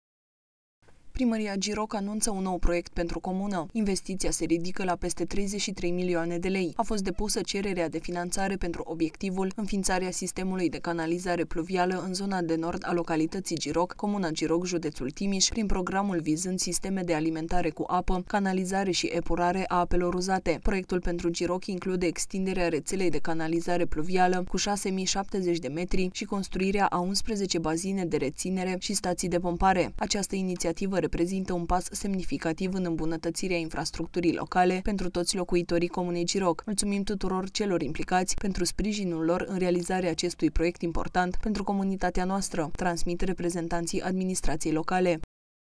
Investiție de peste 33 milioane de lei în Giroc
Primăria Giroc anunță un nou proiect pentru comună. Investiția se ridică la peste 33 de milioane de lei. A fost depusă cererea de finanțare pentru obiectivul „Înființarea sistemului de canalizare pluvială în zona de nord a localității Giroc, comuna Giroc, jud. Timiș”, prin programul vizând sisteme de alimentare cu apă, canalizare şi epurare a apelor uzate. Proiectul pentru Giroc include extinderea rețelei de canalizare pluvială cu 6.070 metri și construirea a 11 bazine de retenție și stații de pompare.